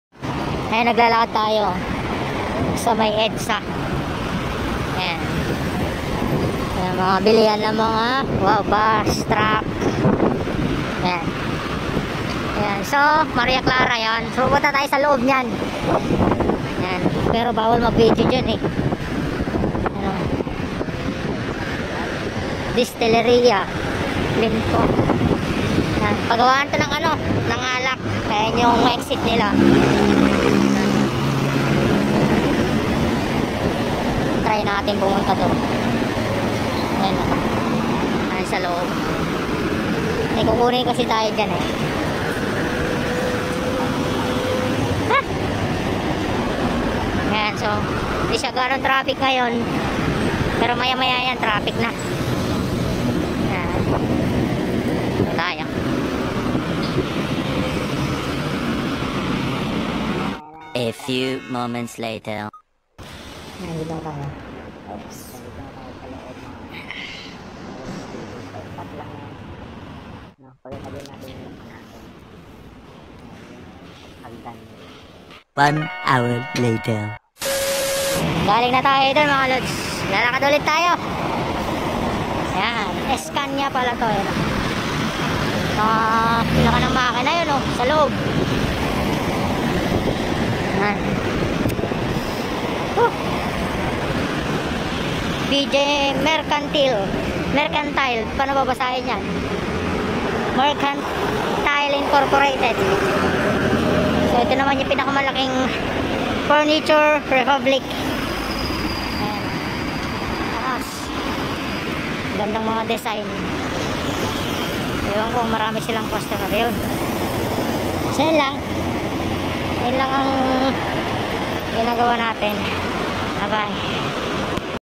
PUMUNTA LANG NAMAN AKO DITO PAGAWAAN NG ALAK PERO BAWAL VIDEO..
Collection time...